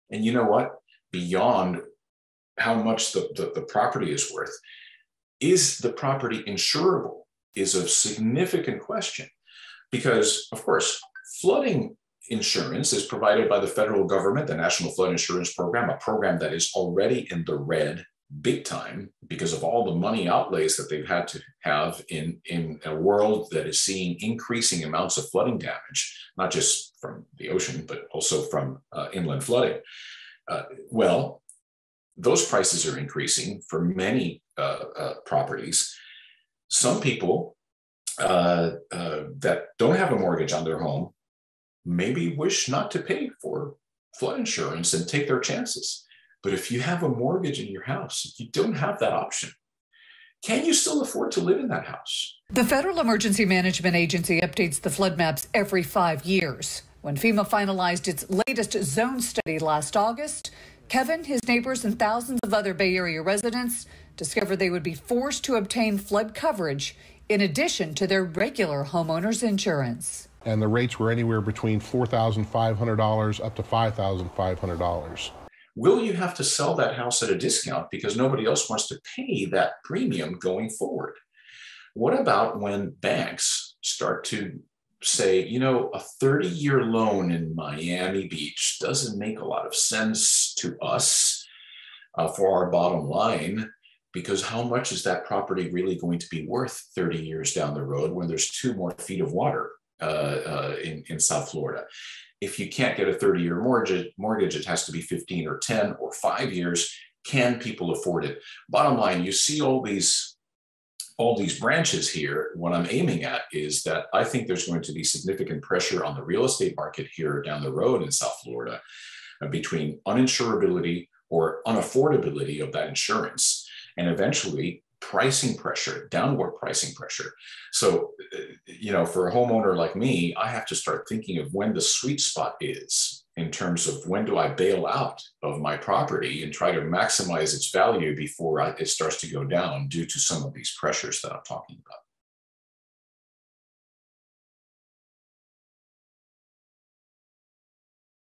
John Morales: Impacts of Florida Flooding on Real Estate
John Morales is Chief Meteorologist of ABC 6 in Miami, Florida.  I had a wide ranging conversation about climate change evidence and impacts in Florida, from a broadcast meteorologist's perspective.